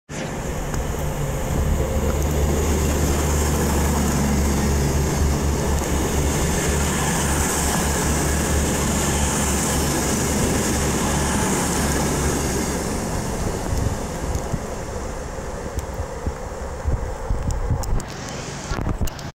A Cross Country Voyager Passes Meadowhall.
A Cross Country Voyager Passes Meadowhall Heading North on Monday 30 November 2015.